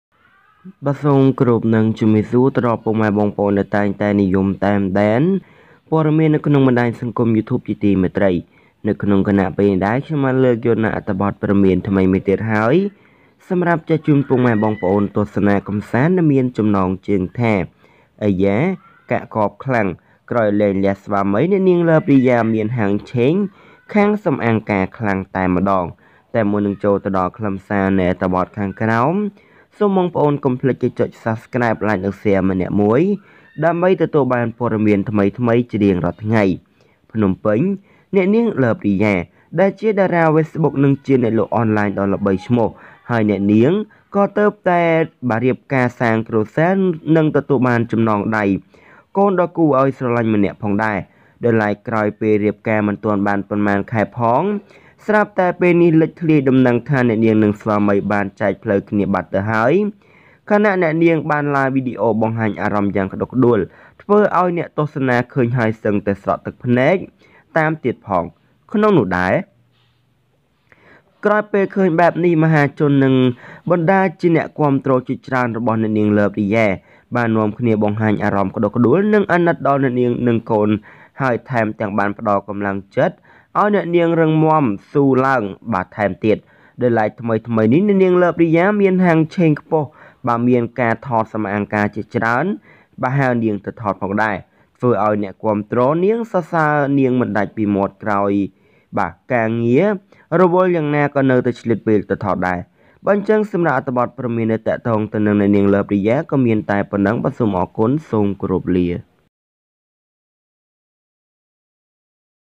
អីយ៉ា! កាក់កបខ្លាំងក្រោយលែងលះស្វាមីអ្នកនាង Love Riya មានហាងឆេងខាងសំអាងការខ្លាំងតែម្ដង (មានវីដេអូ)
Wow!  Love Riya divorces' after 'divorce'

Sombon kh #